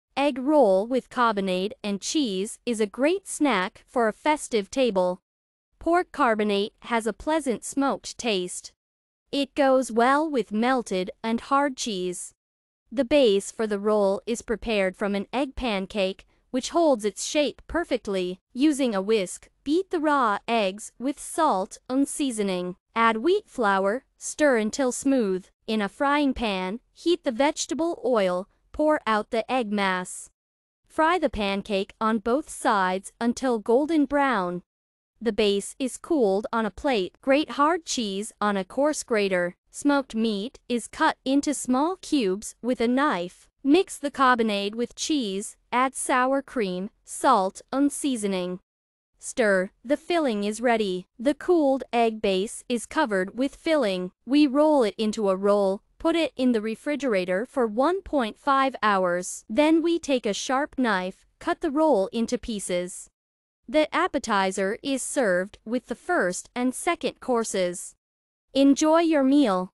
Egg roll with carbonate and cheese
Egg roll with carbonade and cheese is a great snack for a festive table. Pork carbonate has a pleasant smoked taste. It goes well with melted and hard cheese. The base for the roll is prepared from an egg pancake, which holds its shape perfectly.

Ingredients:
chicken eggs - 3 pcs, carbonate - 50 g,
hard cheese - 50 g,vegetable oil - 2 tbsp.l.,
wheat flour - 1 tbsp.l., salt - to taste,